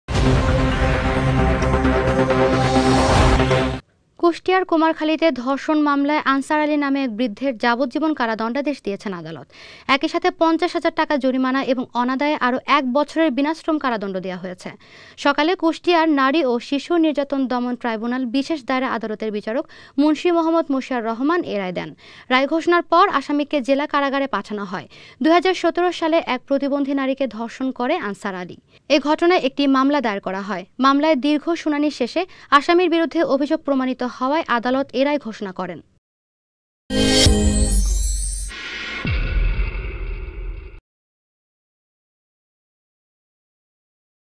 কুমারখালীতে ধর্ষণ মামলায় বৃদ্ধের যাবজ্জীবন কারাদণ্ডাদেশ | Kustia | Kumarkhali
কুষ্টিয়ার কুমারখালীতে ধর্ষণ মামলায় আনসার আলী নামে এক বৃদ্ধের যাবজ্জীবন কারাদণ্ডাদেশ দিয়েছেন আদালত। একই সাথে ৫০ হাজার টাকা জরিমানা ও অনাদায়ে আরো এক বছর বিনাশ্রম কারাদণ্ড দেয়া হয়।
সকালে কুষ্টিয়ার নারী ও শিশু নির্যাতন দমন ট্রাইব্যুনাল বিশেষ দায়রা আদালতের বিচারক মুন্সী মো: মশিয়ার রহমান এ রায় দেন। রায় ঘোষণার পর আসামিকে জেলা কারাগারে পাঠানো হয়। ২০১৭ সালের এক প্রতিবন্ধী নারীকে ধর্ষণ করে আনসার আলী। এ ঘটনায় একটি মামলা দায়ের করা হয়। মামলায় দীর্ঘ শুনানি শেষে আসামির বিরুদ্ধে অভিযোগ প্রমাণিত হওয়ায় আদালত এ রায় ঘোষণা করেন।